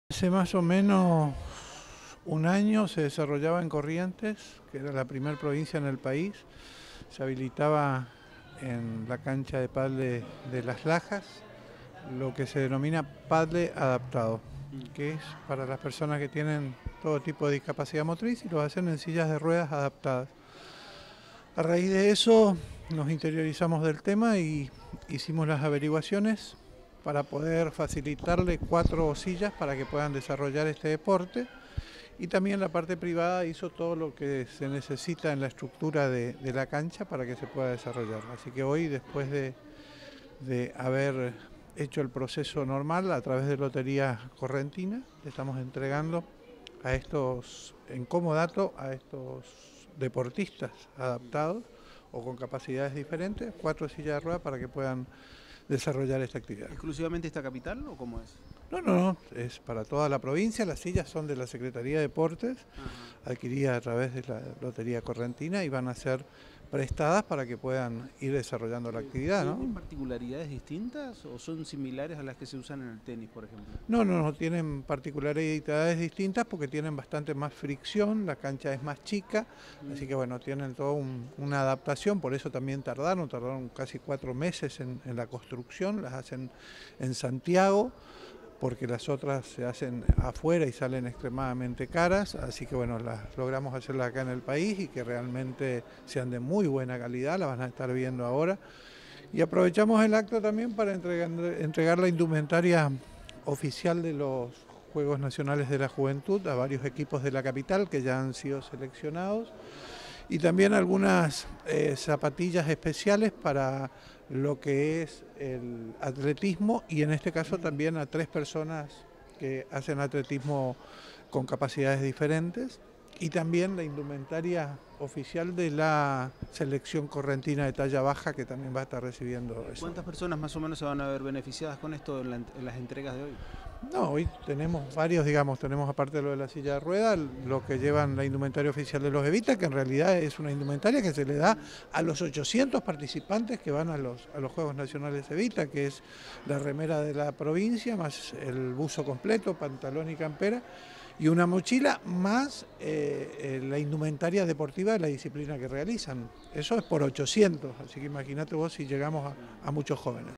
LA PROVINCIA ENTREGÓ SILLAS DE RUEDAS, INDUMENTARIAS Y SUBSIDIOS AL DEPORTE ADAPTADO
En el Salón Verde el Gobierno de la provincia, a través del Ministerio de Desarrollo Social y de la Secretaría de Deporte, realizó la entrega de subsidios, elementos deportivos y sillas adaptadas.
Corrientes es la primera provincia del país que práctica paddle adaptado, que es realizado por deportistas de diferentes discapacidades. Gracias a la inversión privada que hizo el Club Las Lajas se adaptaron las canchas para que puedan ser utilizadas. En este contexto, la Secretaría de Deportes entregó sillas de ruedas adaptadas para la práctica de paddle adaptado a Juan Eduardo Maciel.
La indumentaria deportiva fue entregada a los representantes de los Juegos Correntinos a realizarse en Mar del Plata 2019, al igual que la Selección Argentina de Talla Baja y los equipos de judo, gimnasia rítmica, gimnasia artística, vóley femenino, y el grupo de atletismo con discapacidad.
El Instituto de Lotería Correntina y Casinos de Corrientes a deportistas del Instituto Superior del Profesorado I-28 de la ciudad de Monte Caseros, de la Fundación Victoria Navajos de Virasoro, el Club Atlético El Porvenir de Esquina, y el Club Atlético Juventud Juventud Unida de Monte Caseros. También recibieron subsidios los atletas Javier Zannino de Bella Vista, Juliana Romero de Capital, y Lautaro Gonzales de Capital.
El ministro de Desarrollo Social Adán Gaya, al dirigirse a los deportistas, manifestó la clara decisión política de “estar al lado de la gente”, teniendo en cuenta que uno de los ejes de gestión del gobernador Gustavo Valdés es la “inclusión, en este caso de la mano del deporte adaptado”.
En tanto, el titular de Deportes, Jorge Terrile, señaló que uno de los objetivos era que estos correntinos dispongan de las sillas de ruedas para pádel y que estas acciones se materializan gracias al aporte de Lotería Correntina.
Quiero felicitarlos a ustedes, que son campeones de la vida, son verdaderos ejemplos y nos enseñan a sortear barreras, demostrando que con ganas todo se puede”, culminó expresando Terrile.
Participaron de la jornada el Ministro de Desarrollo Social, Adán Gaya; el Secretario de Deportes, Jorge Terrile; la Coordinadora del Consejo de Discapacidad, Roxana Tanurí; la interventora del Instituto de Previsión Social (IPS), Lorena Lazaroff; y el jefe de la Policía de Corrientes, Félix Barbosa.
AGRADECIMIENTO POR EL MATERIAL AL CANAL 5TV DE SUDAMERICANA.